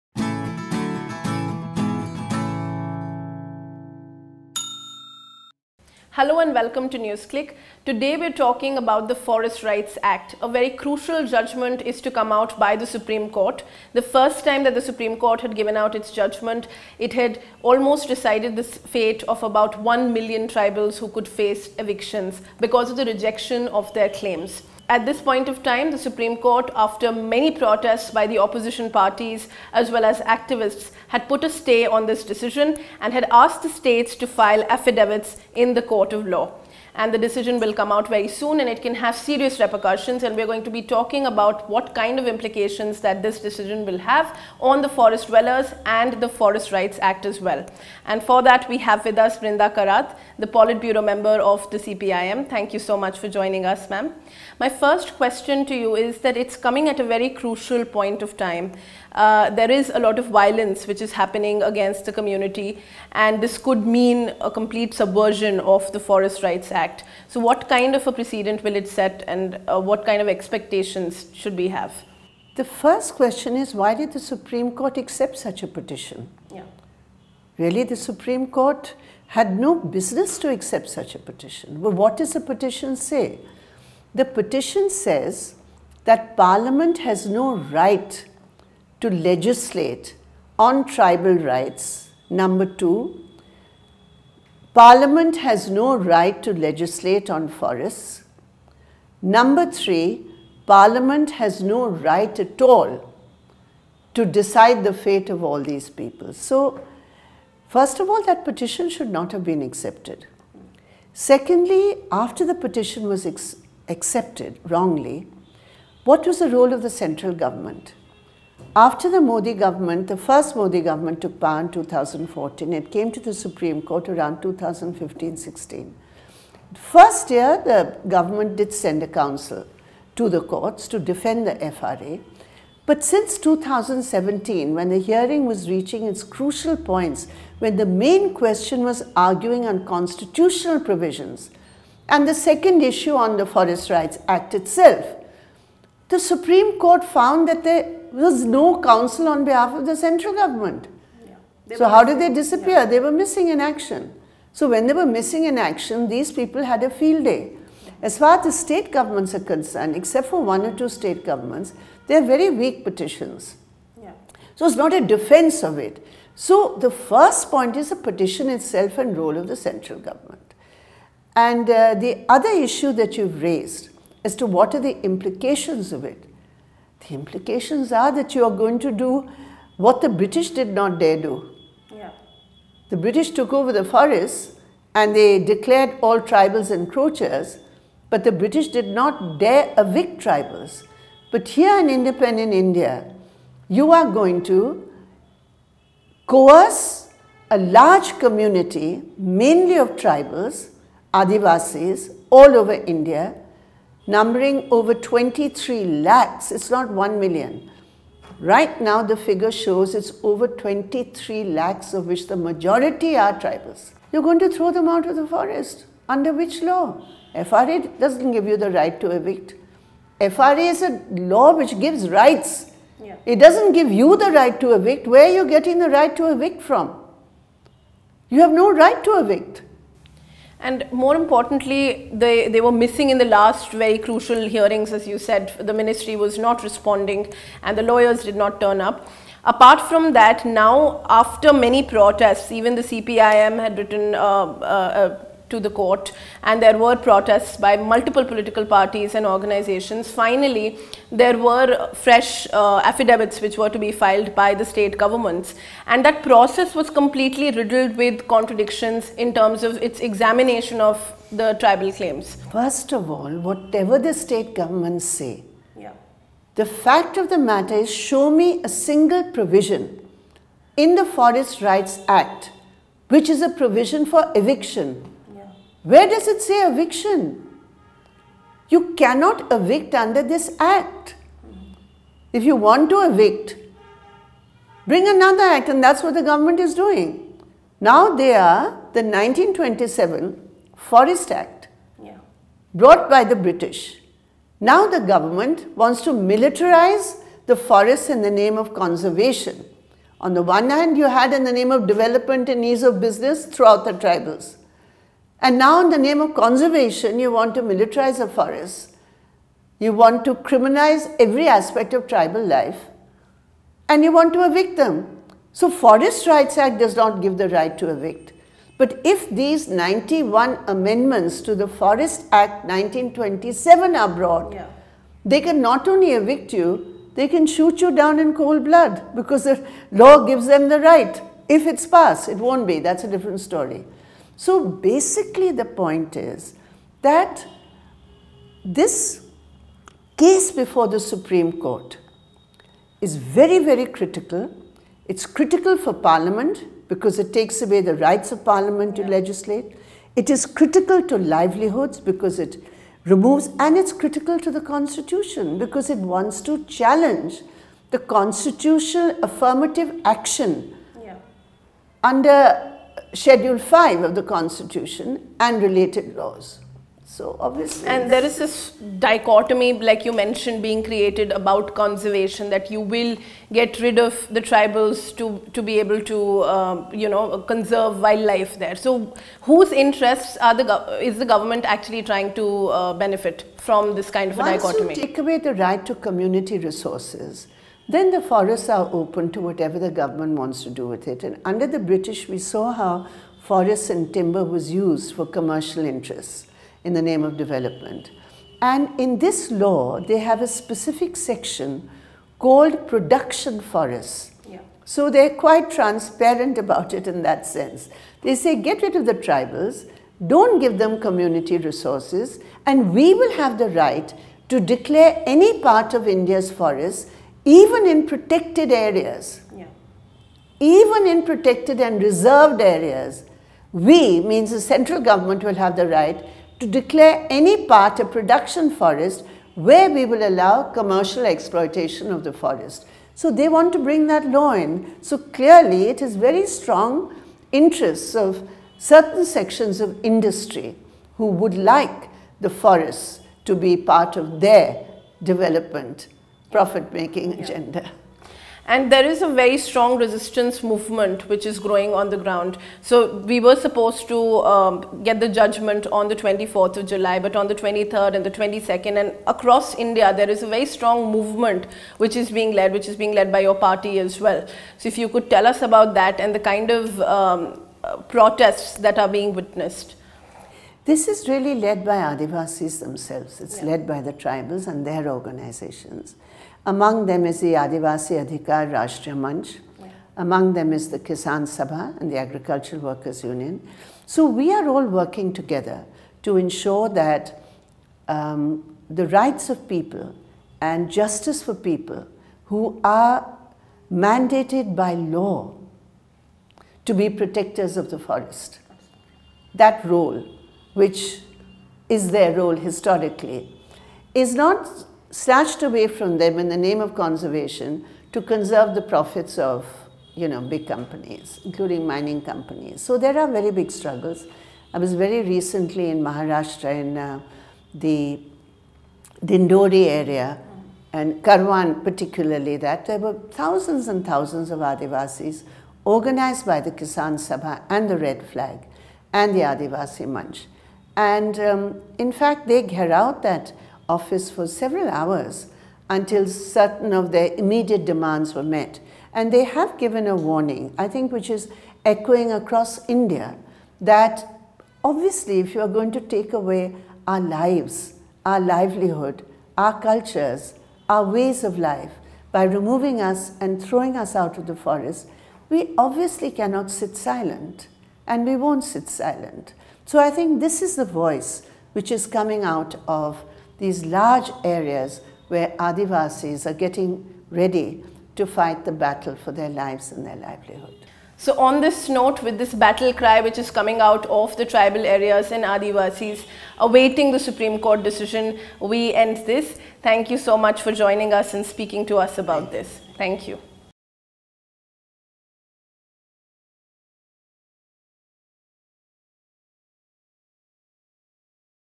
Brinda Karat: Misuse of FRA for Benefiting Corporates?
The Supreme Court is going to decide on the fate of over one million tribals who are facing possible eviction following rejection of their claims to the land. Brinda Karat, Polit Bureau member of the Communist Party of India (Marxist), speaks about the consequences of the petition in the Supreme Court and the validity of the case.